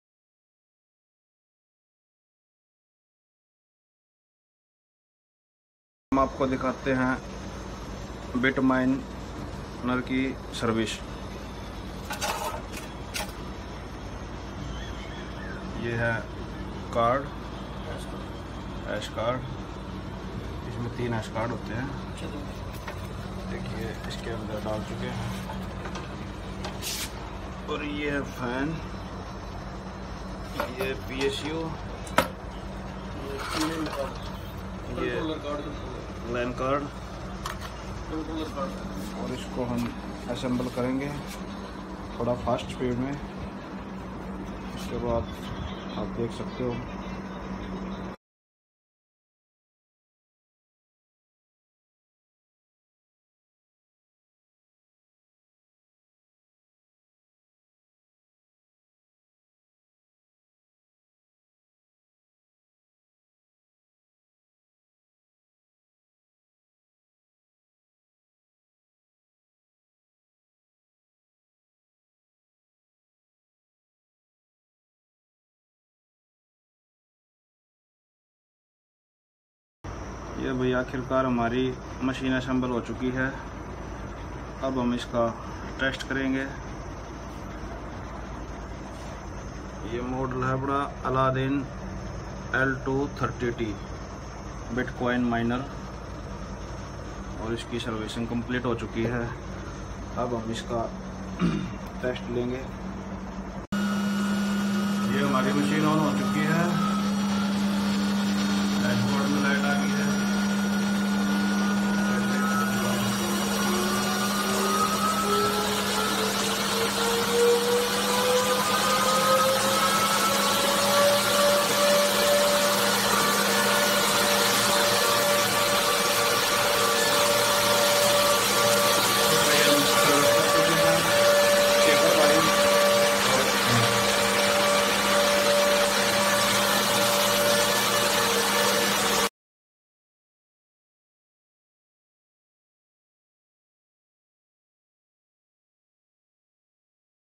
Bitcoin Antminer Service
Many people ask about the preparation of the ASIC miner for immersion cooling. And many believe this is a complicated process. The truth is that not complicated at all. Even used, dusted air-cooled miners require just a little bit of cleaning. We show 5 simple steps for proper preparation of ASIC miner:
1) remove the fans, covers, panels from ASIC miner and PSU if needed
2) remove dust with compressed air.
3) clean each of the hash boards and housing with isopropyl alcohol. Use soft, long-hair brush for the hash boards. Housing can be cleaned also using an ultrasonic cleaner.
4) repeat the process until miner components pass the white glove test.
5) assemble everything and use fan spoofers or immersion firmware Done! You are ready for the immersion cooling! New miners require just retrofitting as described in
1) and some compressed air. If you have the option you can immerse the miners finally in a larger container full of IPA and shake it to remove all final residues. Please remember about health and safety when working with IPA. You need good ventilation, we advise respiratory protection when working with evaporating alcohol and make sure there is no open fire around.